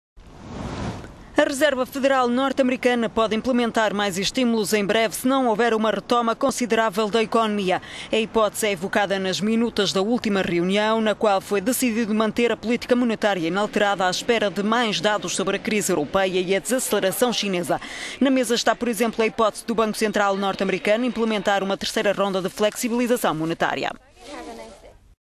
Minutas: FED está pronta a agir em breve
A Reserva Federal norte-americana (FED) pode implementar mais estímulos em breve se não houver uma retoma considerável da economia. A hipótese é evocada nas minutas da reunião a 31 de julho e 1 de agosto. Na altura, a política monetária ficou inalterada, à espera de dados adicionais sobre a crise na Europa e a desaceleração da China.

Na mesa está, por exemplo, a hipótese do banco central norte-americano implementar uma terceira ronda de flexibilização monetária. A taxa de juro diretora está, desde finais de 2008, perto de zero.